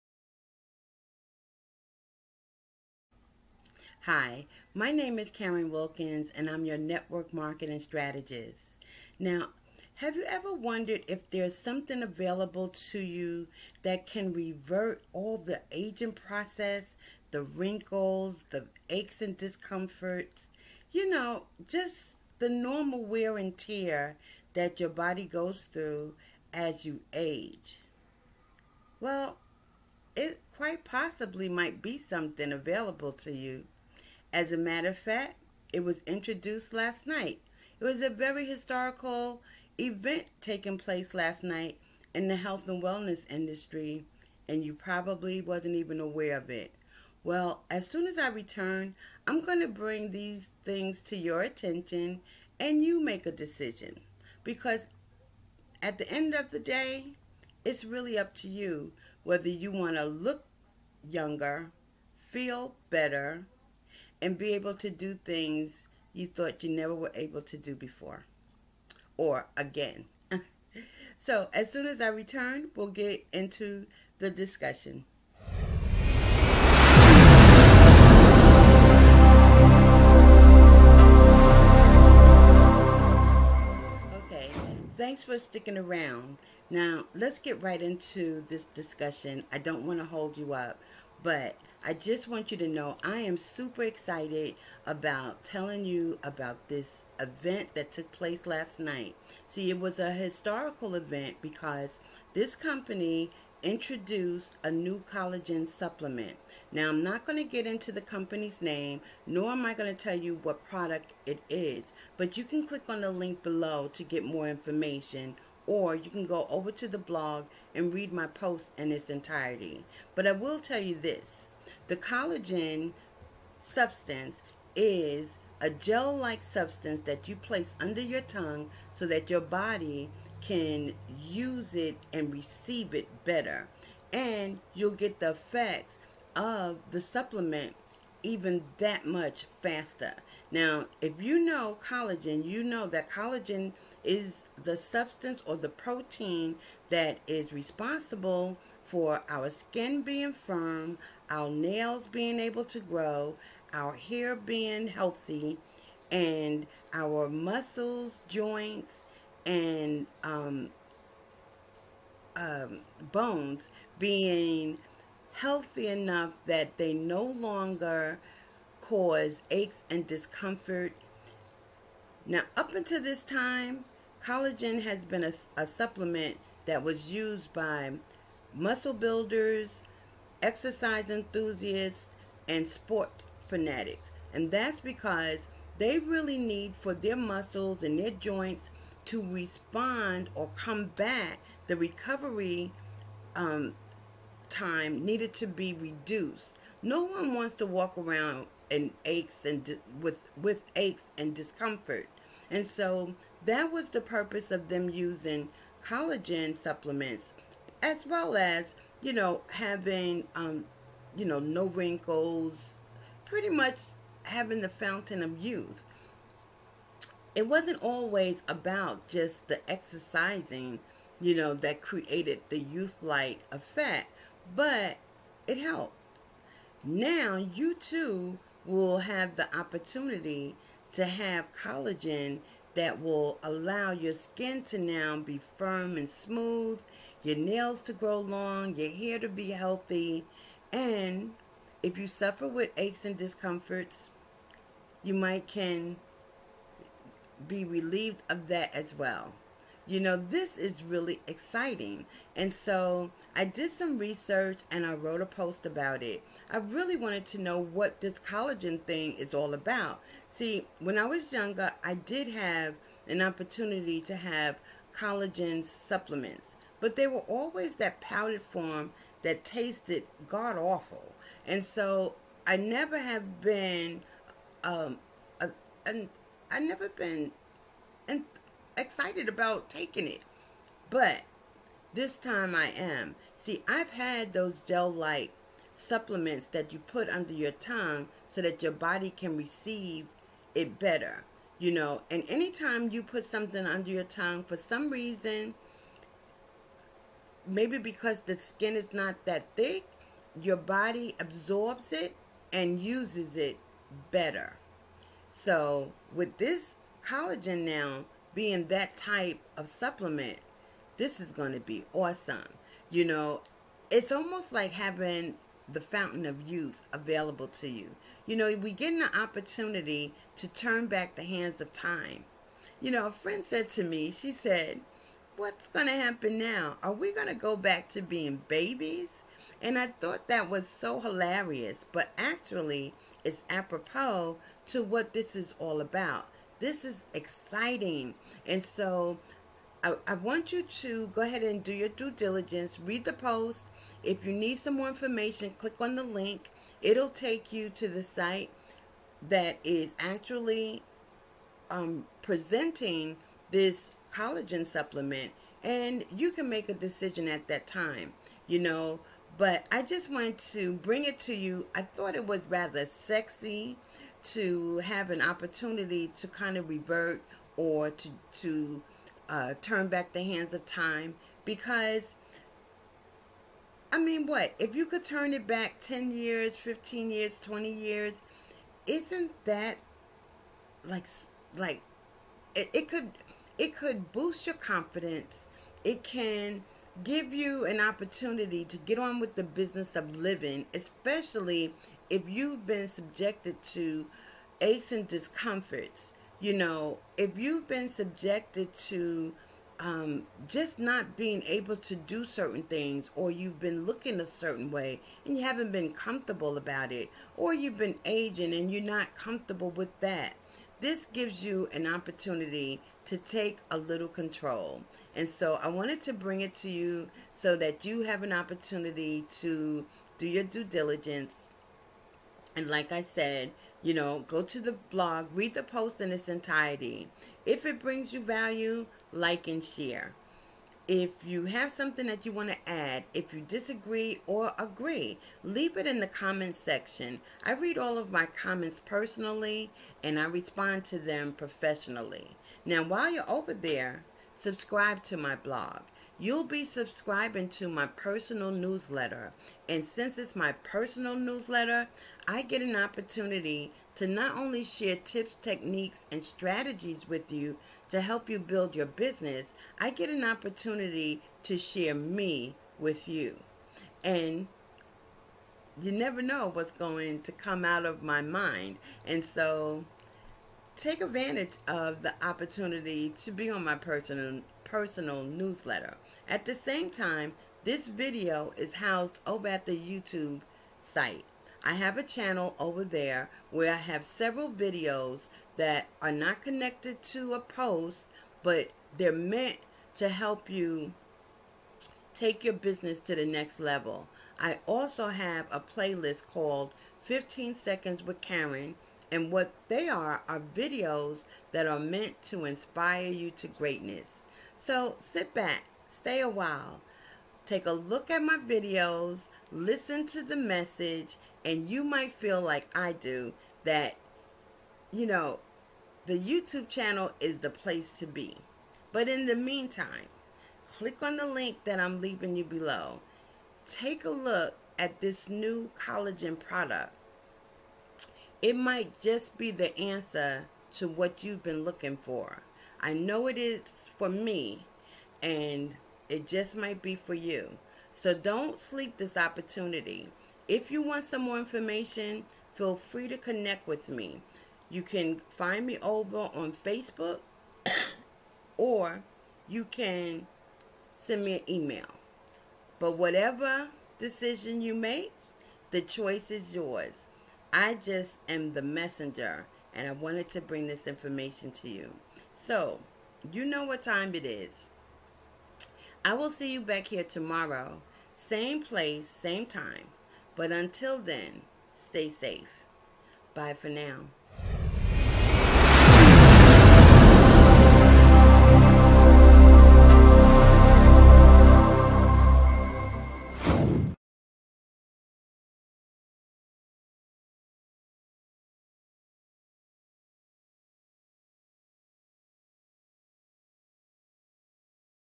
Collagen is Not Just For.....
In recent years, studies have shown that collagen has been a favorite supplement of body builders, exercise enthusiast and some sport fanatics.  Mainly, because collagen is very effective and reducing the muscle’s recovery time after a workout and in repairing tissue and muscle breakdown due to “wear and tear” due to age.
Recently, however, Collagen has even been found in some lotions and vitamins as an added ingredient, to reverse wrinkles, sagging skin and the deteriorating effects the loss of Collagen has on your joints, muscles, nails, hair and etc.

What is Collagen
Collagen is the most abundant protein in our bodies. It’s found in our muscles, bones, skin and tendons. It’s the gel-like substance, some refer to as the “glue”, that helps hold the body together. It gives our skin strength and elasticity, and helps replace dead skin cells.
The production of collagen in our body’s begin to slow down around the age of 30.  Because of this, we begin to experience, wrinkles, sagging skin, weakened finger nails and weaker cartilage in your joints.  This process is accelerated with smoking, high sugar intake and long exposure to the sun.

1.  Collagen is good for the Hair, Nail, Skin and Teeth
2.  Collagen is good for the Metabolism and Energy
3.  Collagen is good for the Joints
4   Collagen taken as a supplement

For More Info on the product that was released and made available go